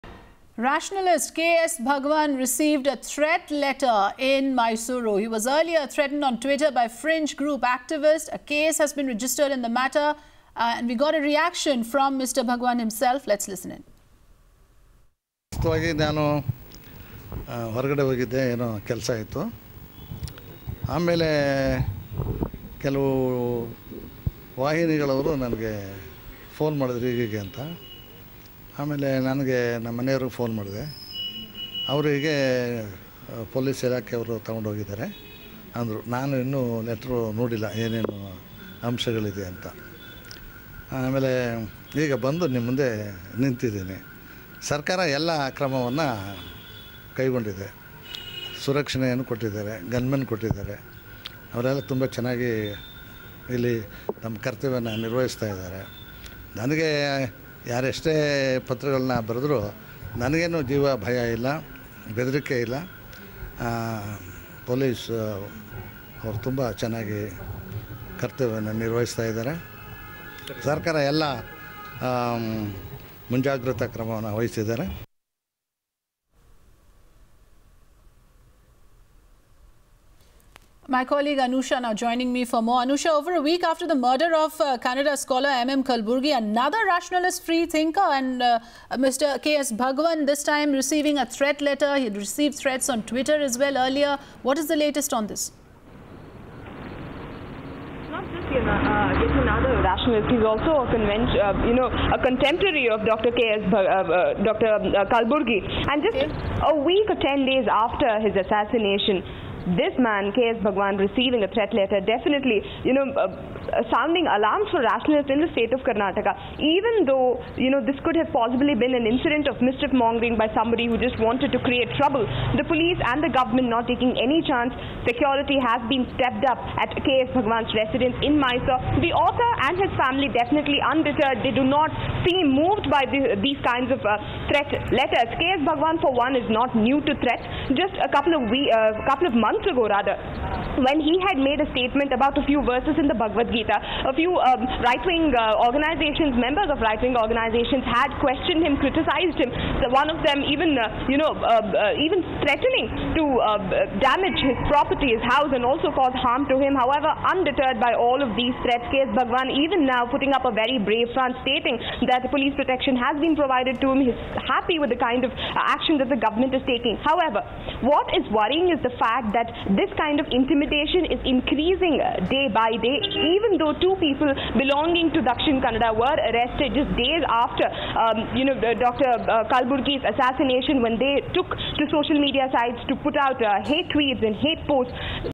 K S Bhagwan receives threat letter
Rationalist KS Bhagwan received a threat letter in Mysore. He was earlier threatened on Twitter by fringe group activist. A case has been registered in the matter.